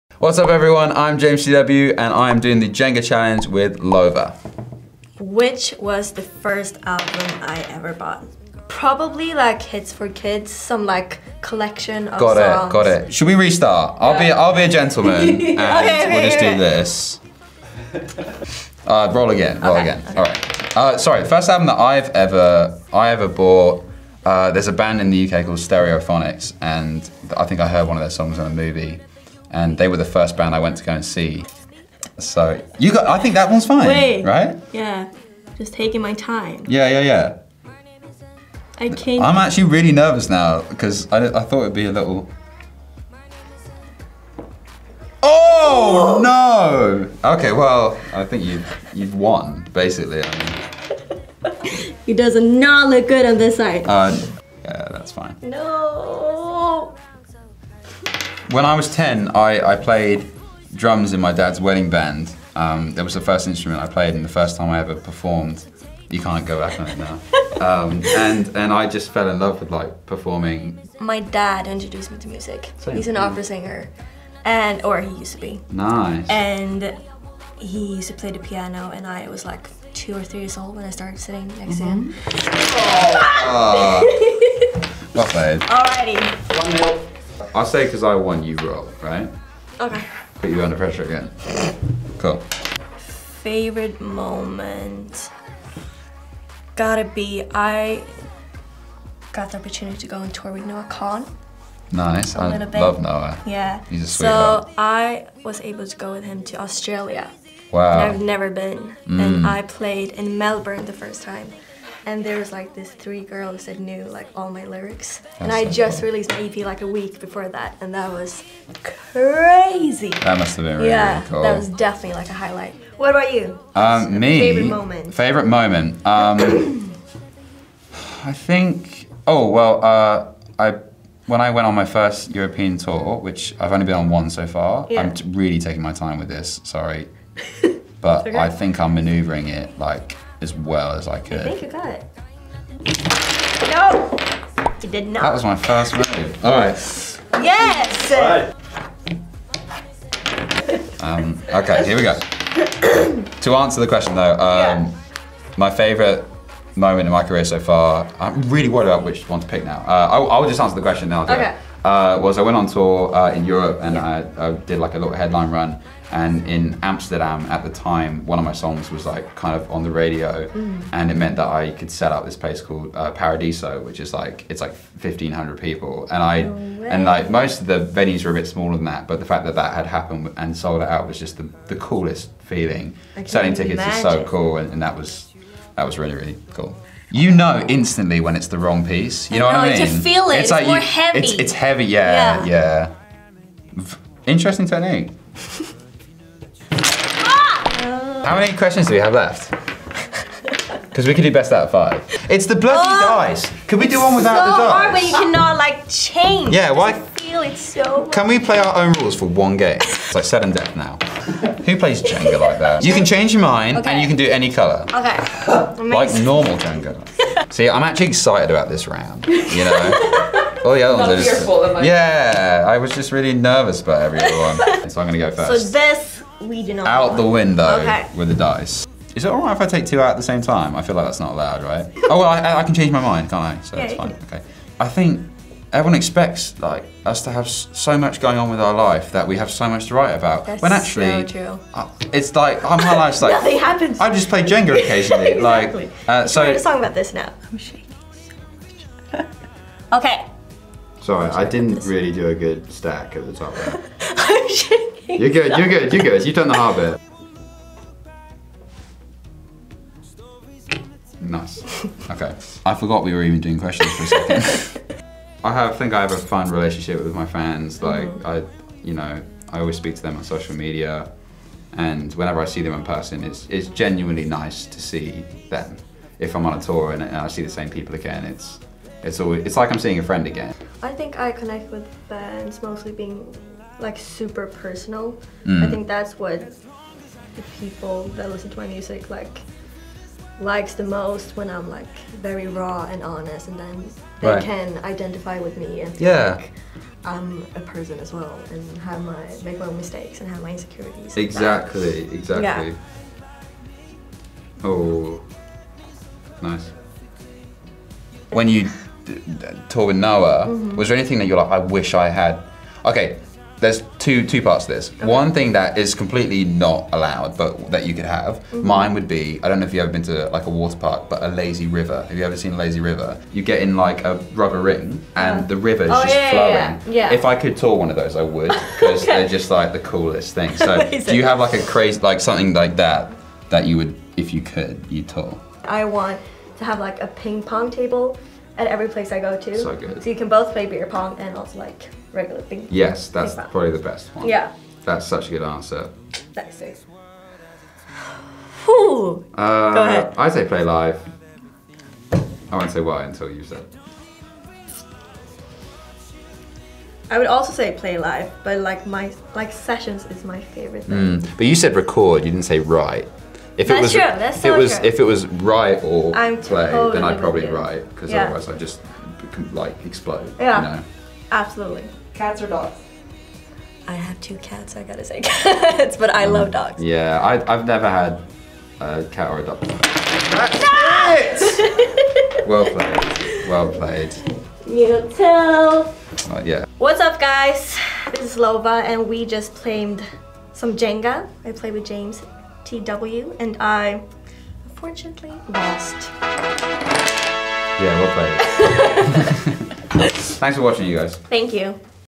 Artists Playing Jenga ft. LOVA & James TW
Watch James TW play Jenga and answer questions with Swedish LOVA!